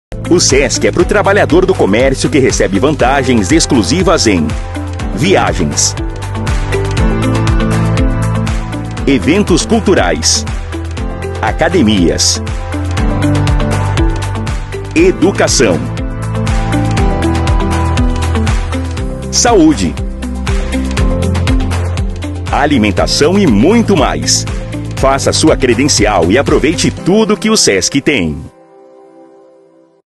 Conheça os Benefícios do Sesc MS
O Serviço Social do comercio tem a missão de educar para a conquista da qualidade de vida sendo referência para os trabalhadores do comércio de bens, serviços e turismo de Mato Grosso do Sul como instituição promotora da qualidade de vida e desenvolvimento social!

No Sesc, o trabalhador do comércio tem vantagens exclusivas que você pode conhecer um pouco melhor nesse vídeo.